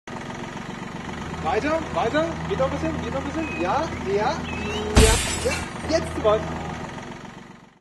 Einweisen will gelernt sein
Einparken ist nicht immer leicht. Wie schön, wenn einem ein Freund hilft. Doch dann...

RofD im Minirock - weitere Kurzfilme ab Sommer 2012 auf

und auf Youtube